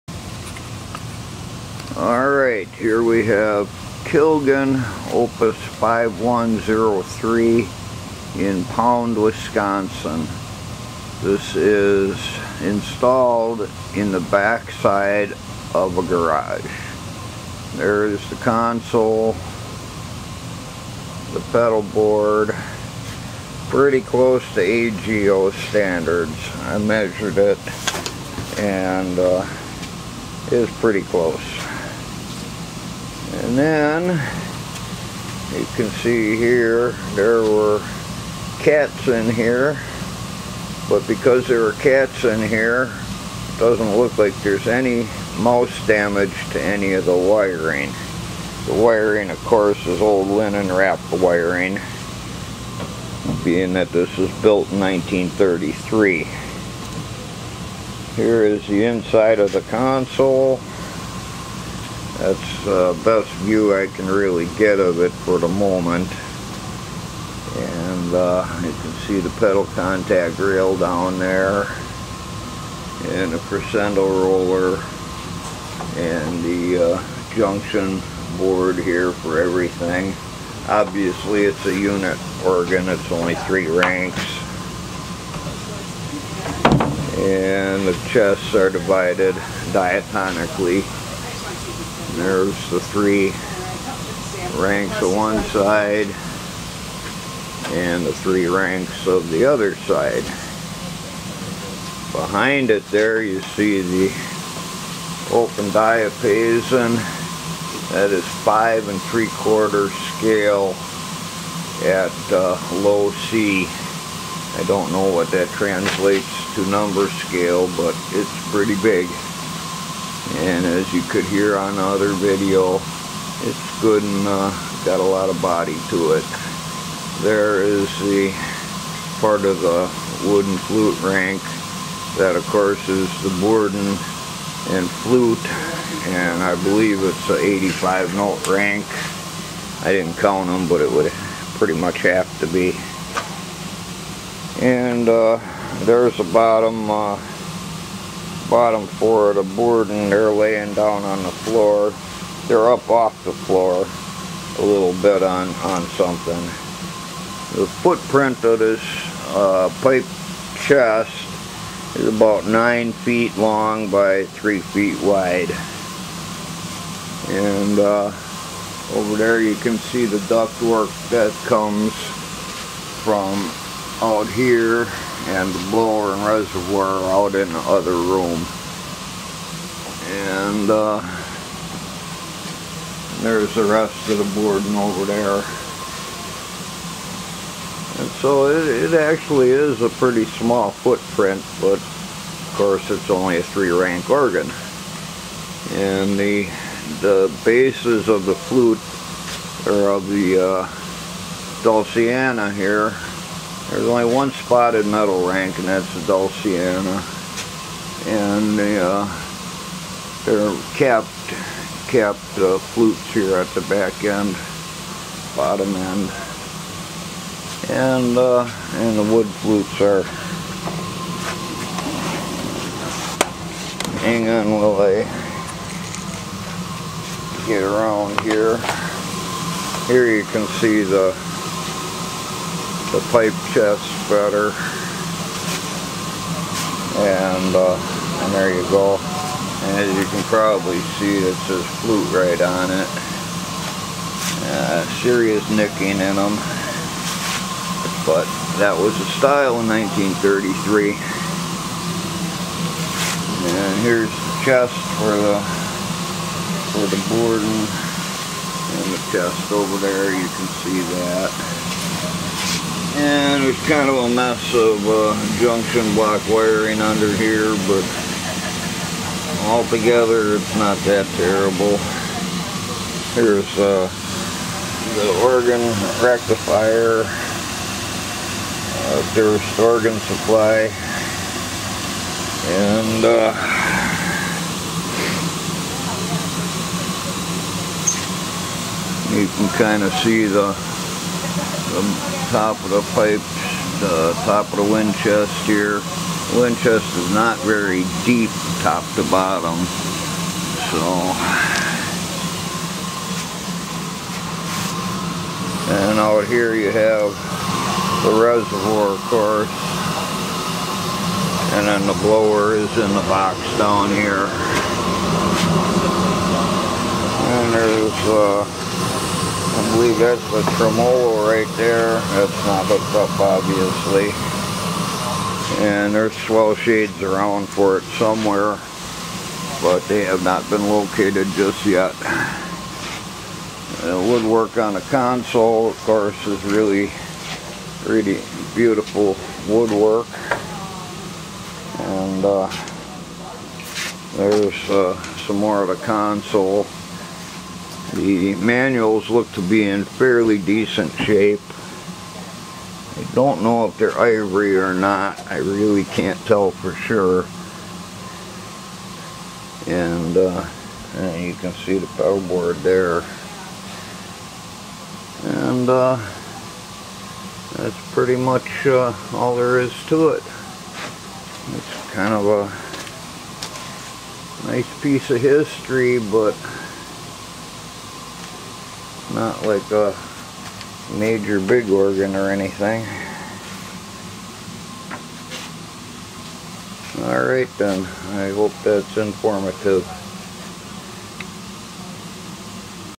Kilgen 3 Rank Unit Pipe Organ, Opus 5103 ( 1933 ), Overview
This is an overview survey of this organ taken on May 1st 2019. The organ was installed in the rear of a two car garage in far northeastern WI. It is in remarkably good condition, considering it's age, and the situation was made to fit into here.

The owner passed away, and the organ needed to be removed so the property could be sold. It was in danger of being simply thrown into a dumpster and trashed.  I rescued it from that fate in May of 2019, and it is here in central WI at my shop, ready for a new owner.

Due care was taken in the removal process, and almost all of the organ has been saved intact.  In between my several trips to the site, the blower was taken by someone else, so it will need one.

The new owner will of course need to install a control system of their choosing, and update the cotton covered wiring as much as needed.  I left plenty of pigtails of the old wiring on the chests and in the console, so tracing and sorting the wiring will be fairly easy.

It would be nice to see this organ find a new home as a complete instrument,  but if it does not get interest from anyone in a reasonable amount of time, it could well end up being parted out.  I do need the space for other projects.

It is a good size for someone who wants a pipe organ in their home, as it does not take that much space.   Or it would make a nice addition to an existing organ, as it is a very good condition 3 rank unit chest with actions for each note.  The leather pouches that I can see ( without taking the chests apart ) look to be in fine condition, and I did not notice any notes Not working during my test play.

In the video I mention that the original swell shades are gone, but I do have a set of shades here that can go with the organ.   It does have a nice swell shade motor that is in decent condition.

If you have any interest in getting this organ, please contact me via e-mail and I can give you more details, photos, etc.   LarryFX1 at yahoo dot com  I will not discuss business particulars via the comments here.

As of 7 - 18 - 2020 this organ is SOLD !  Thank you John J.   I'm sure you will give it a good home, and I look forward to checking it out in it's next life.